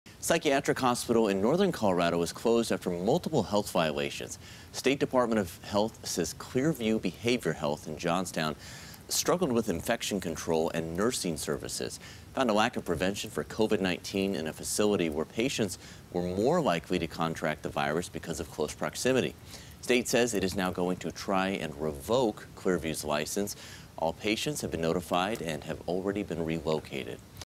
Multiple Health Violations Reported At Clear View Behavioral Health
The state is working to revoke the psychiatric hospital's license.